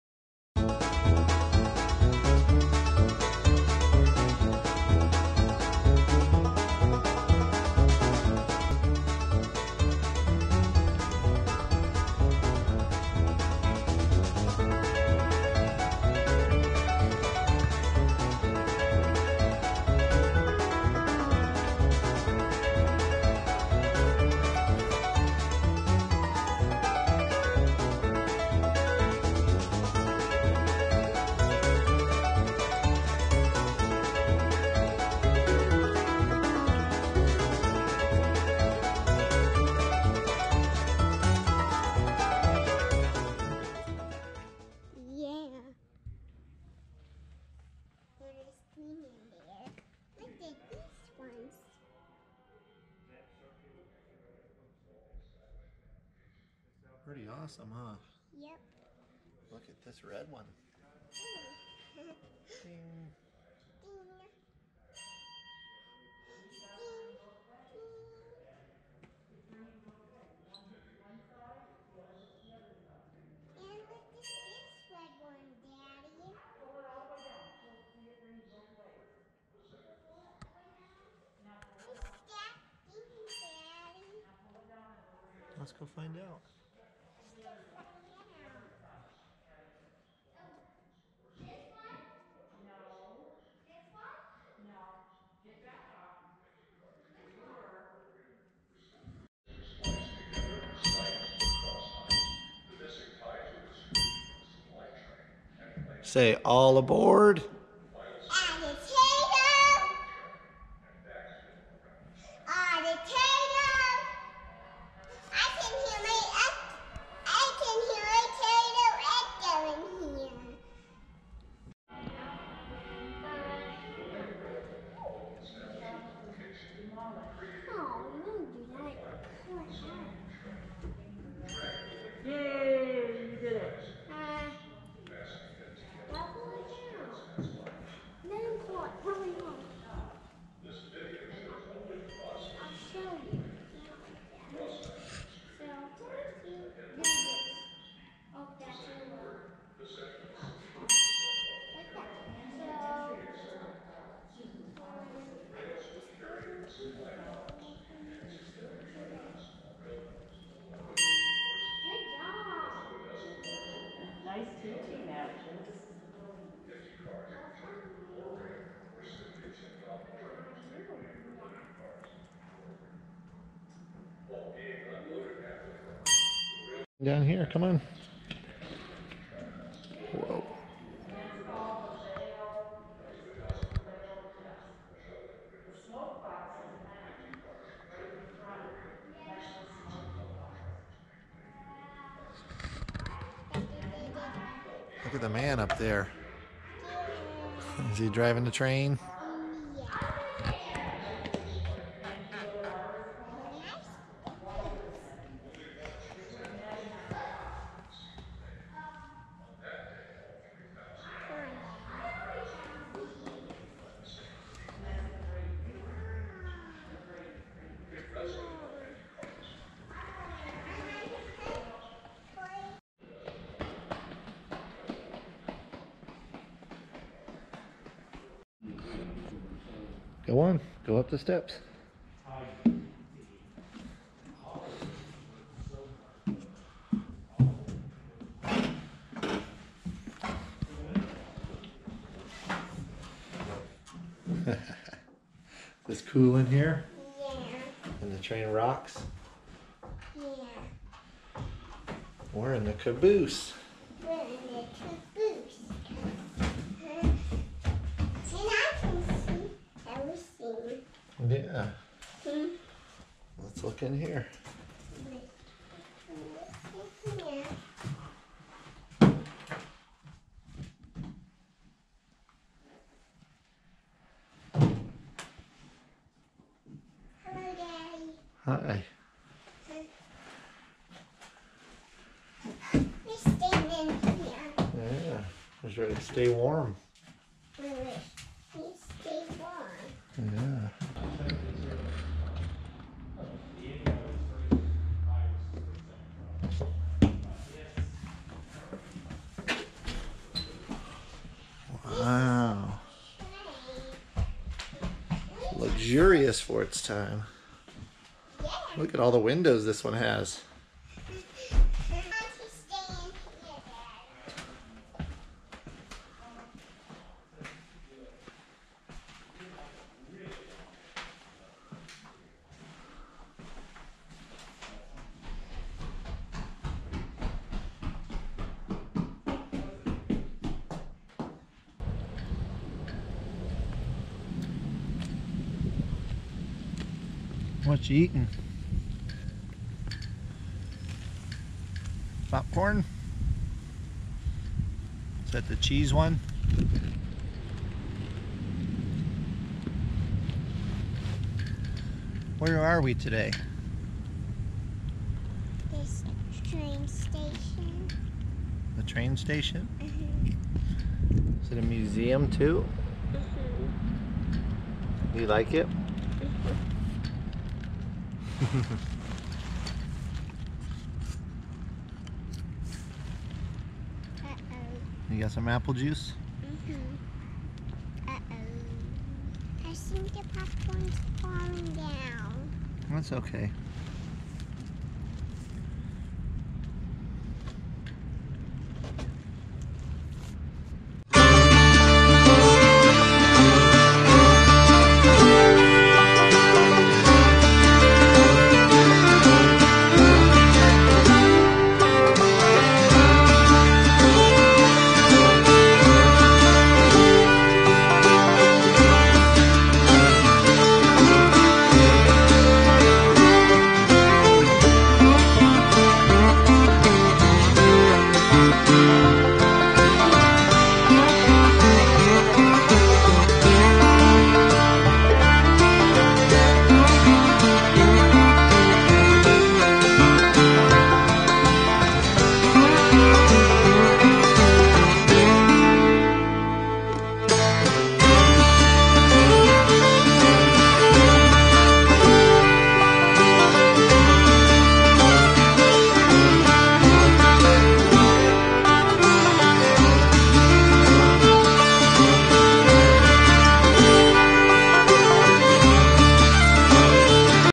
Nevada State Train Museum
There are some great historical train cars, Locomotives, Caboose, and rail cars here to explore amongst models and things for the whole family to enjoy. Train rides available select weekends.